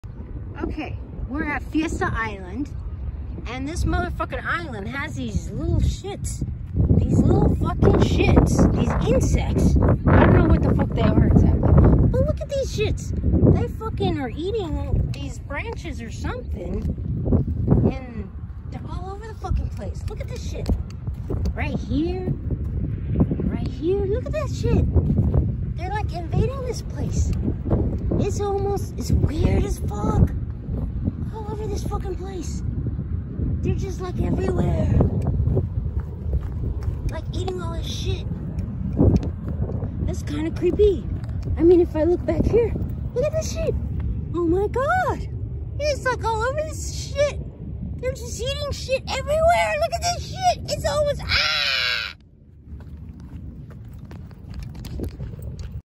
Fiesta Island Doggo Park Fucking Red Bug Infestation
These are my inner thoughts on these creepy crazy fucking red bugs Bebe and I saw in the doggo park…